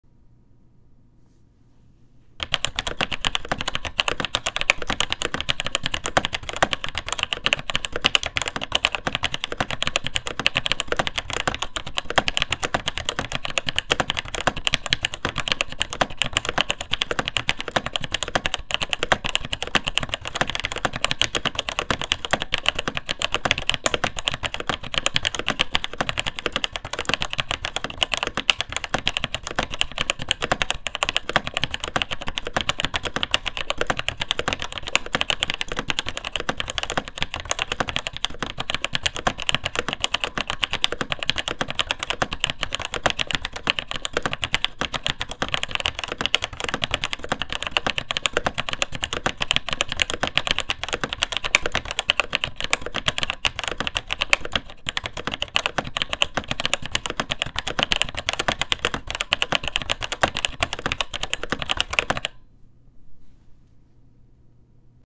Taiwan Jet Axis Switch | Vintage Mechanical Keyboard Sound Test | ASMR
white / clear / kind of bluish variant

linear switch, feels kind of sludgy and slow but not necessarily scratchy... it's weird, but they do clack pretty hard

they remind me of the MEI T-15 series switch found in datalux spacesaver keyboards in both sound and keyfeel

this keyboard had a literal piece of paper instead of a plate... i took it out because it made the keyboard feel and sound terrible, not that it feels great now

NOVA KEY 101 branded Unitek K-251
fcc id: DKW67MK-256

this keyboard shares an fcc id with another seemingly more common keyboard, Unitek K-260